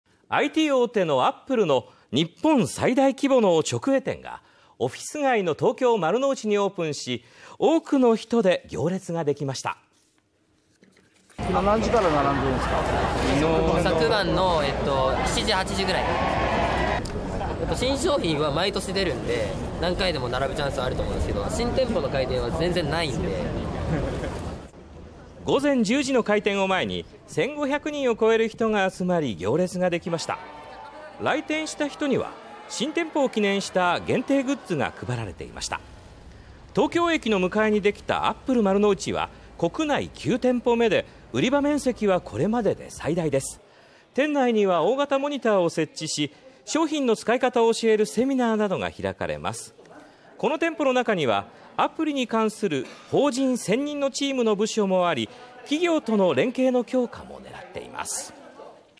「アップル」国内最大規模の直営店 開店前に1500人(19/09/07)
IT大手「アップル」の国内最大規模の直営店がオフィス街の東京・丸の内にオープンし、多くの人で行列ができました。　開店前に並んだ人：「（Q.何時から並んでいる？）昨晩の7時から8時くらいからです。新商品は毎年出るので何回でも並ぶチャンスあるが、新店舗の開店は全然ないので」　7日午前10時の開店を前に1500人を超える人が集まり行列ができました。来店した人には新店舗を記念した限定グッズが配られていました。東京駅の向かいにできた「アップル丸の内」は国内9店舗目で、売り場面積はこれまでで最大です。店内には大画面のモニターを設置し、商品の使い方を教えるセミナーなどが開かれます。丸の内の店舗にはアプリに関する法人専任のチームの部署もあり、企業との連携強化も狙っています。